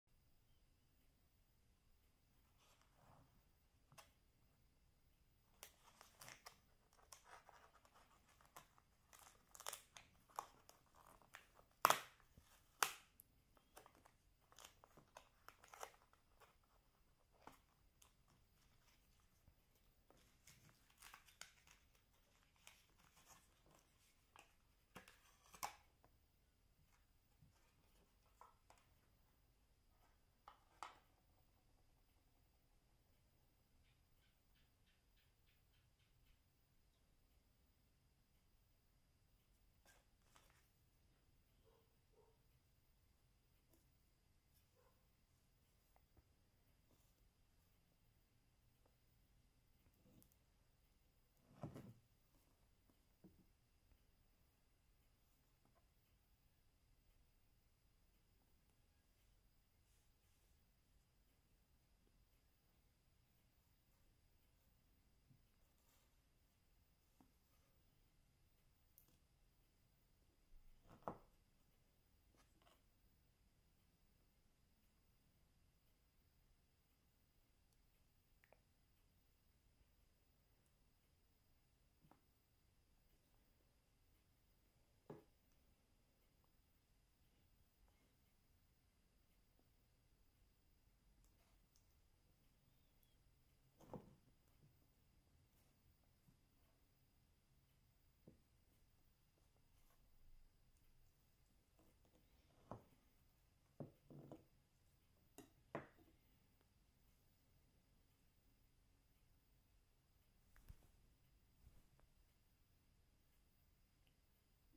Fruit cake cutting ASMR Sennheiser Ambeo Smart test with iPhone 12 pro max
First time of my new microphone
Very excited and surprise that I got it at Chiang Mai Thailand

Listen and enjoy with your earphone